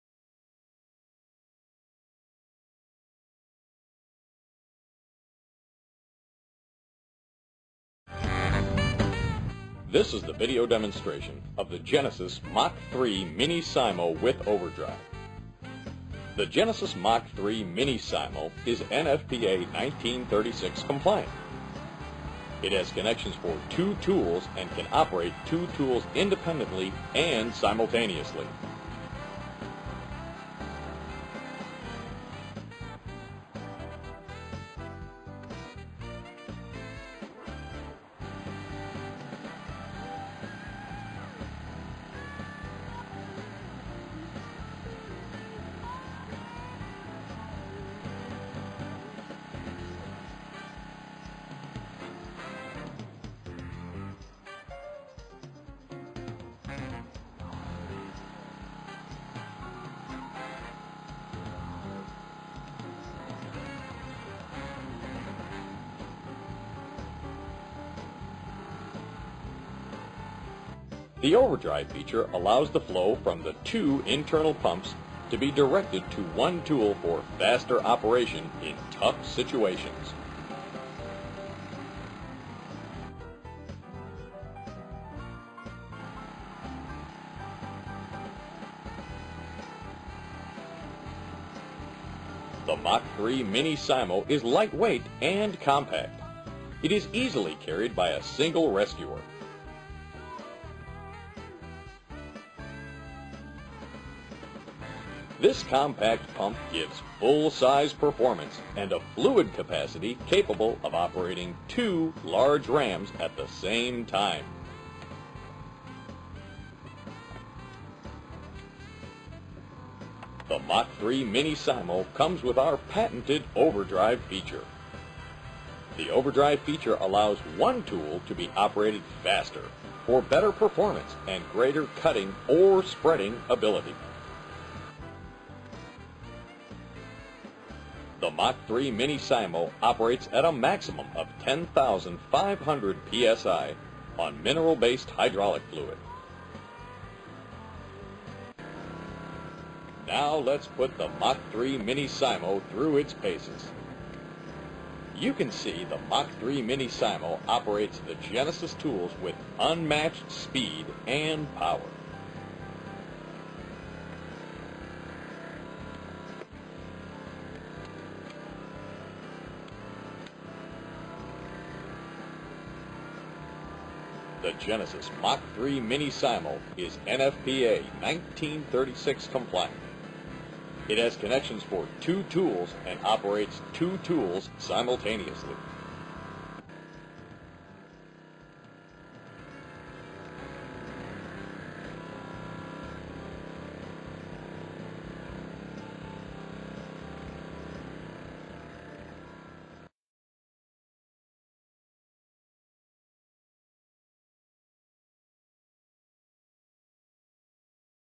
Genesis Hydraulic Rescue Tool Pump from Nassau Fire Apparatus
This video demonstrates the use of a Genesis Mach III Mini Simo hydraulic pump operating two different hydraulic rescue equipment extrication tools simultaneously. The pump has a patented overdrive feature that allows one tool to be used at twice the speed, giving the tool more power to cut or spread.